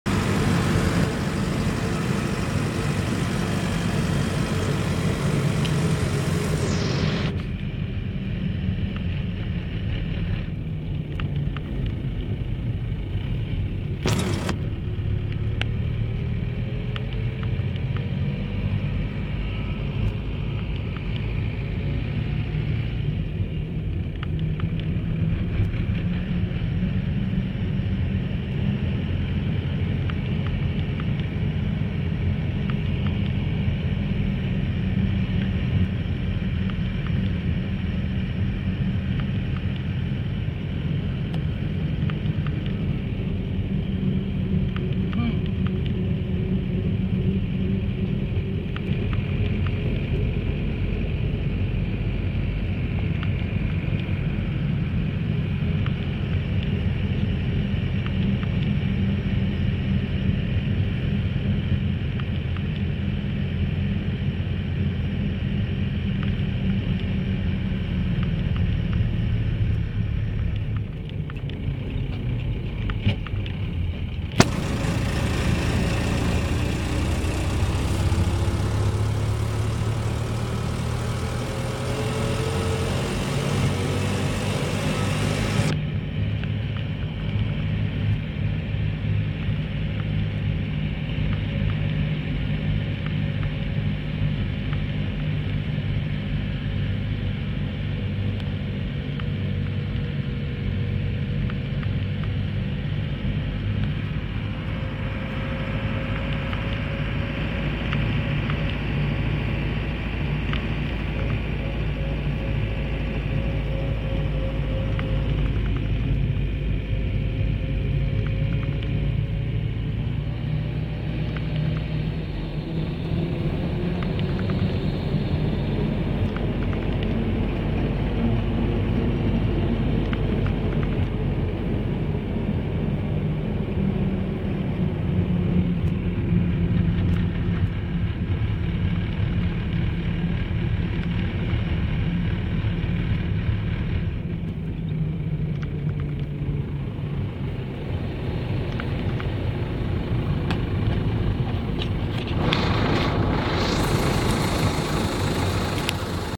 Traveling to Philippines countryside Vid #2
This road is newly paved about 2 yrs old. this will lead you to Bucari known as little Baguio in Iloilo City Philippines.